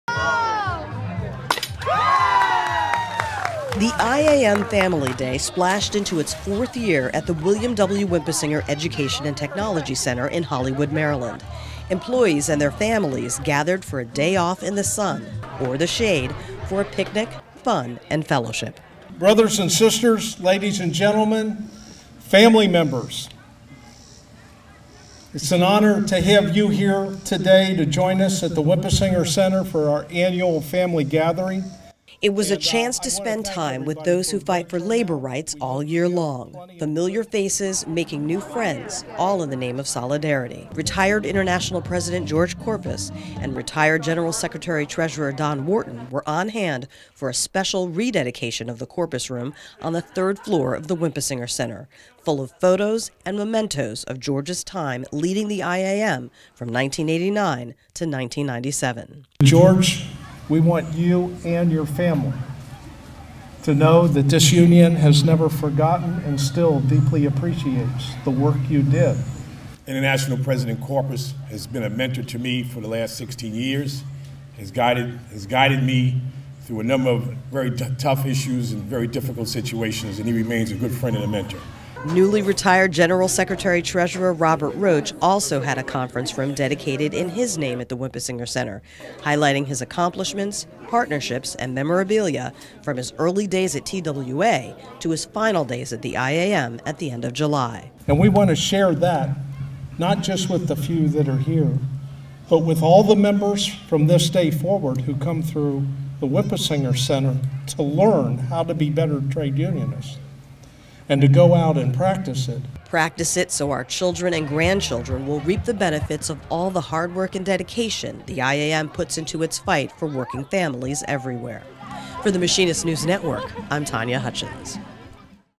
IAMAW Family Day
For the fourth year, the IAM hosted a Family Day Picnic for employees at the William W. Winpisinger Education & Technology Center, where two rooms were dedicated in honor of recently retired General Secretary-Treasurer Robert Roach, Jr. and retired International President George Kourpias. Retired GST Don Wharton was also on hand for the events.